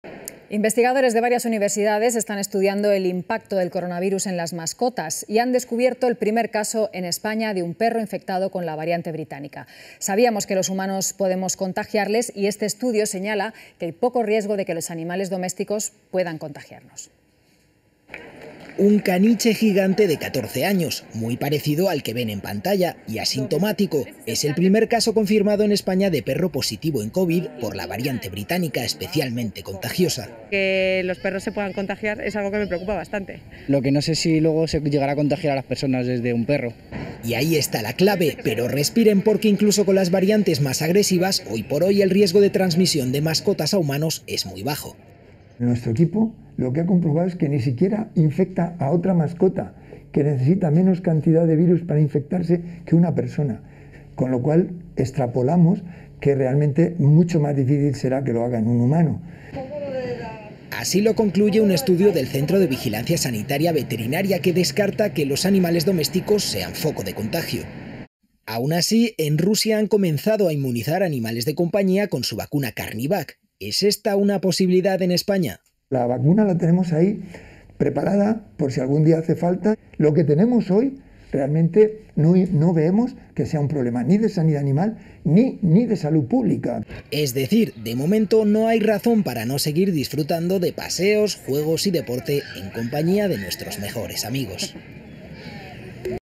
COVID EN MASCOTAS: el riesgo de infección es muy BAJO | RTVE NOTICIAS
Investigadores de varias universidades están estudiando el impacto del coronavirus en las mascotas...y han descubierto el primer caso en España de un perro infectado con la variante británica. El estudio señala que hay poco riesgo de que los animales domésticos puedan contagiar